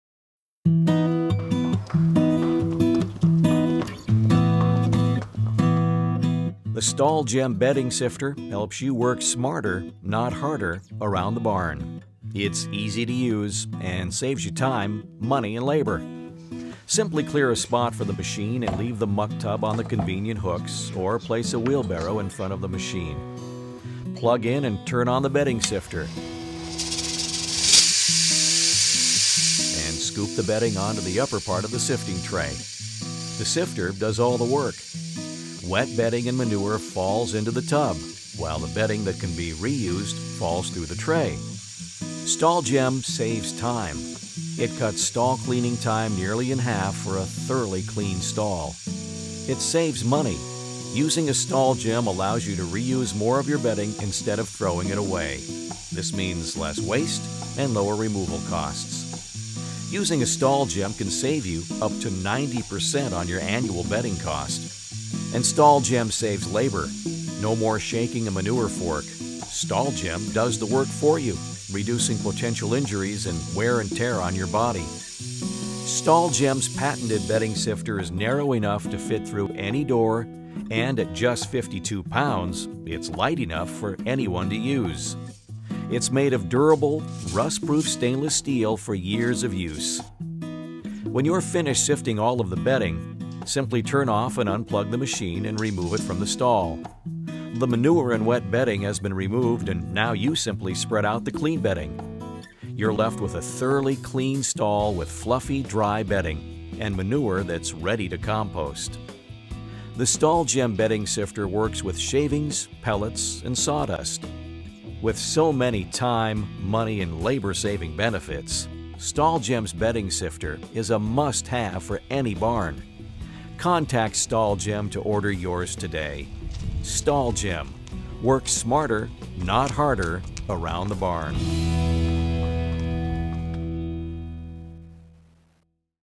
StallGem Bedding Sifter
StallGem Mfg. Co. American made horse stall bedding sifters. Made of rust-resistant stainless steel (powder coated mild steel model shown). A must-have product for horse owners that use wood savings for horse bedding. Also works with sawdust and pellets. Patent No. US 7673755 B2.
* Makes Stall Cleaning Easier
* Improves Horse Health & Comfort
* Makes Manure Composting Possible
* Prevents Injuries
* Saves Time & Bedding

* Want cleaner stalls? Checkout our website!